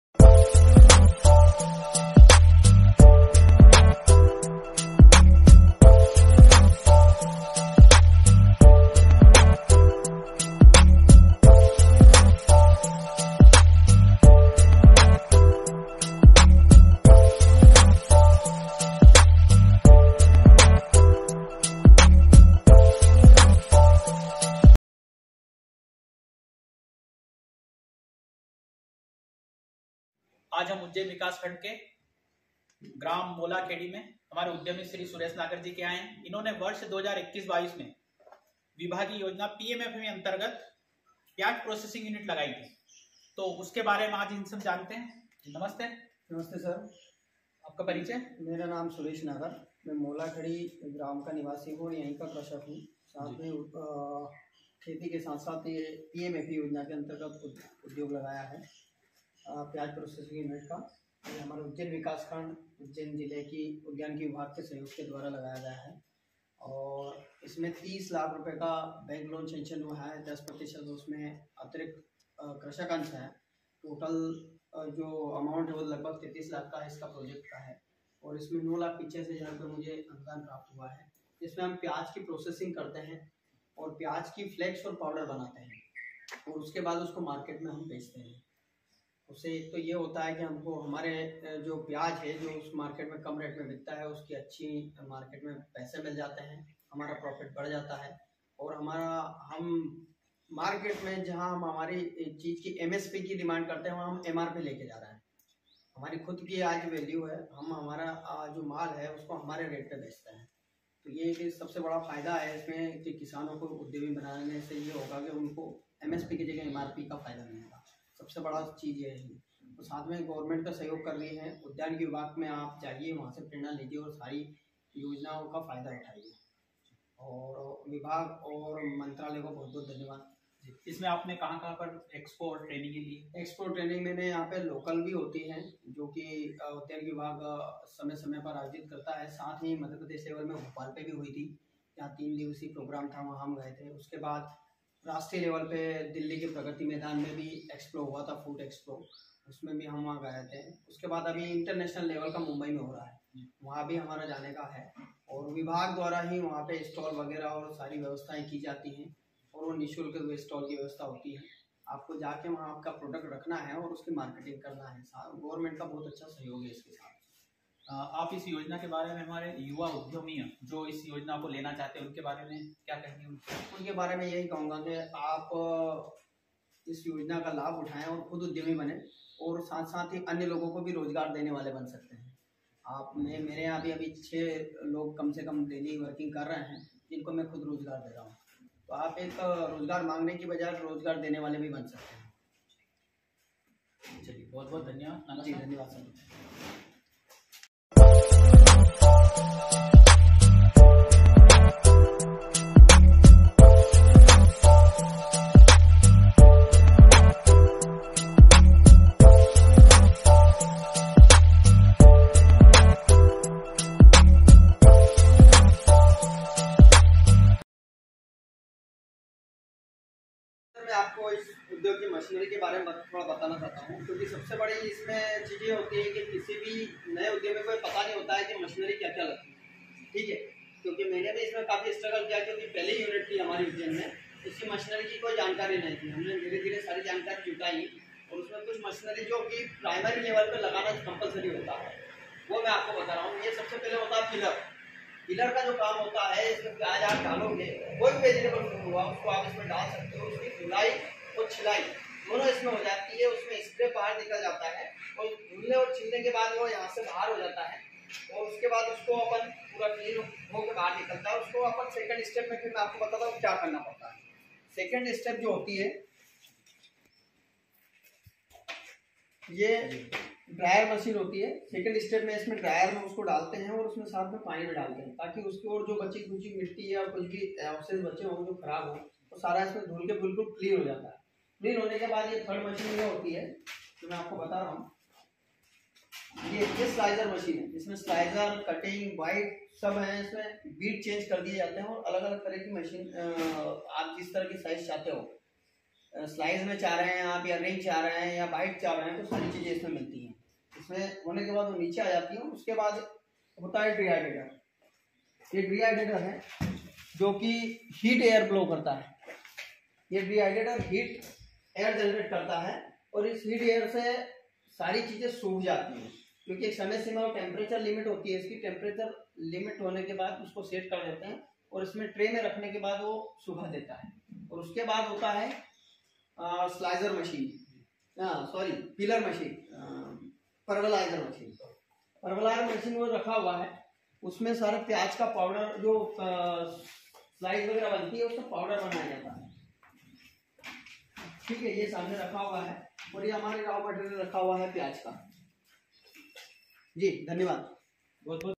प्याज प्रोसेसिंग यूनिट जिला उज्जैन
हितग्राही : सुरेश नागर, ग्राम - मोलाखेड़ी, विकासखंड - उज्जैन, जिला - उज्जैन
उत्पादन - प्याज पाउडर, प्याज स्लाइस,